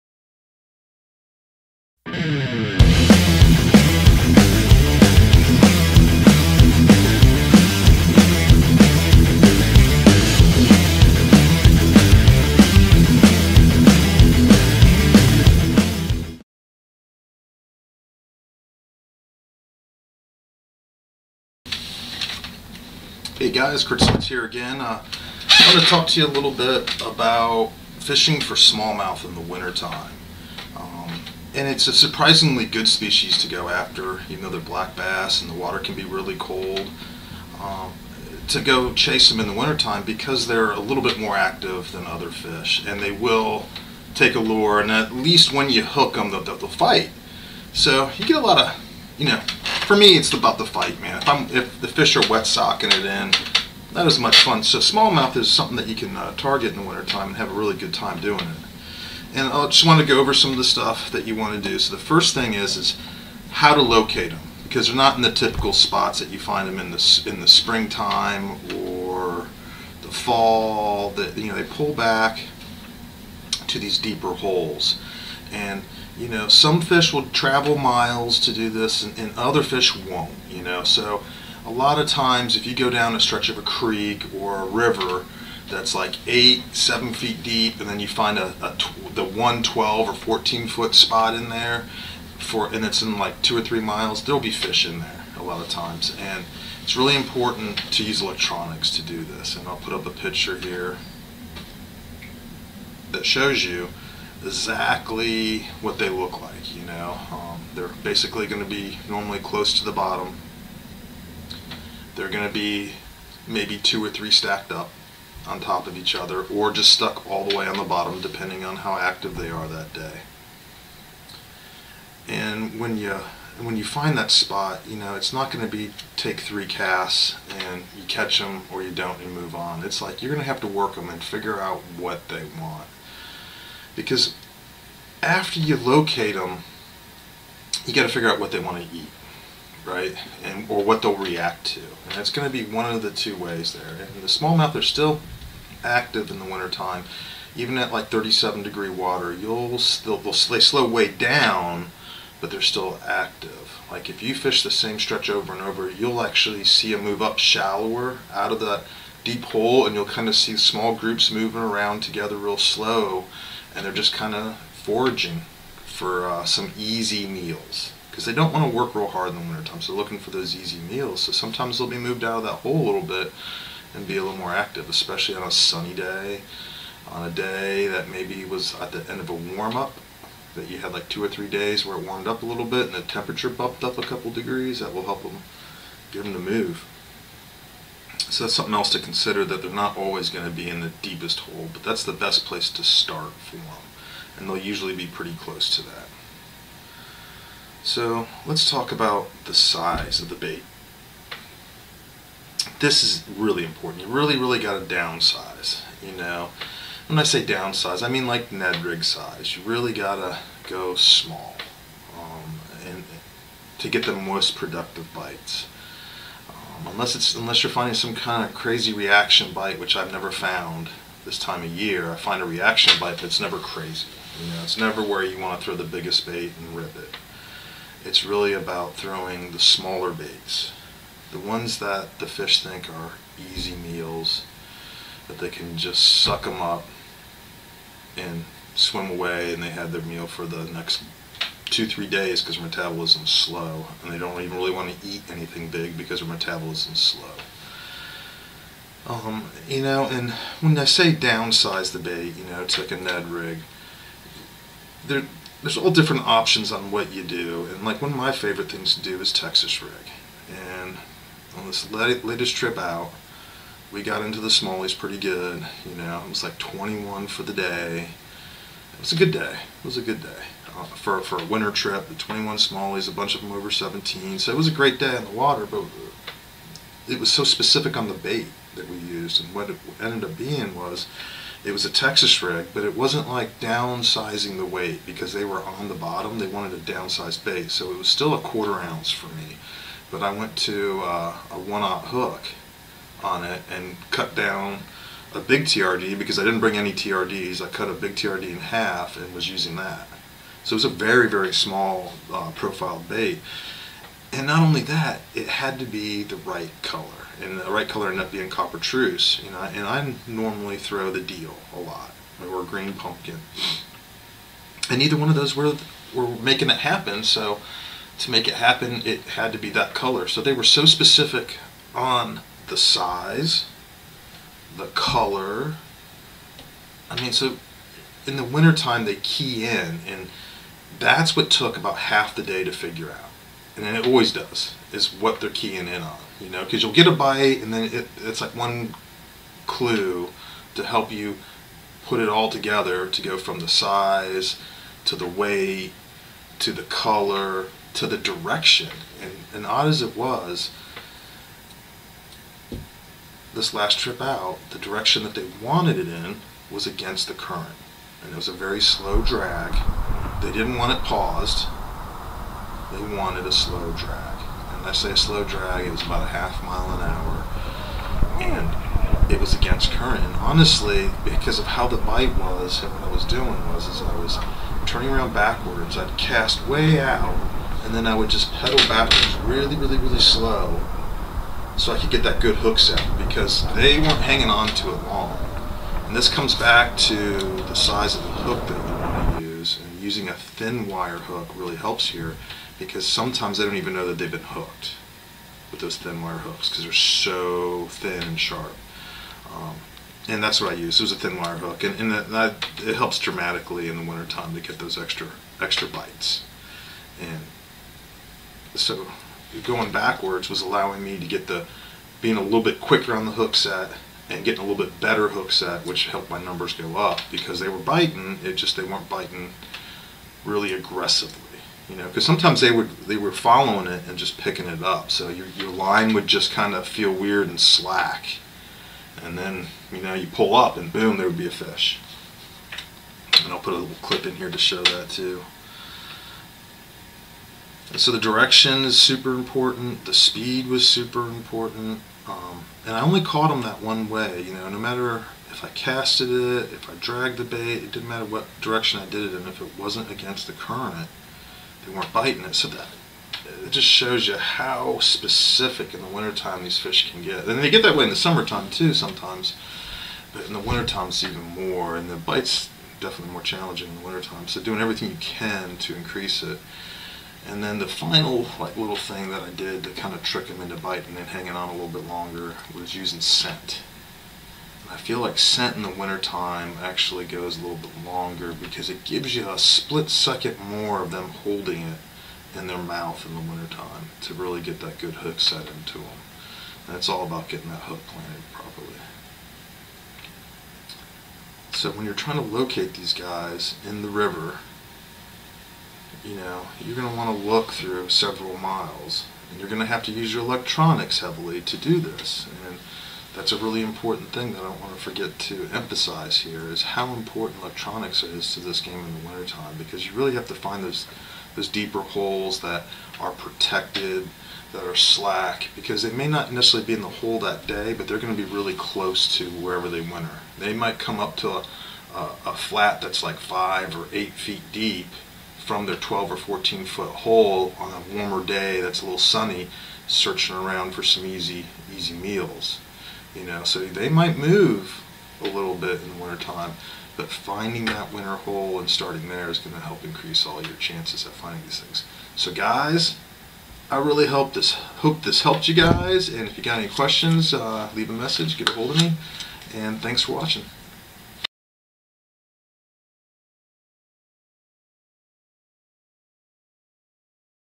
Tips on catching cold water smallmouth bass
Going over how to approach catching cold water smallmouth bass. Winter can be intimidating time of year to chase smallmouth but it can be one of the best time to catch numbers.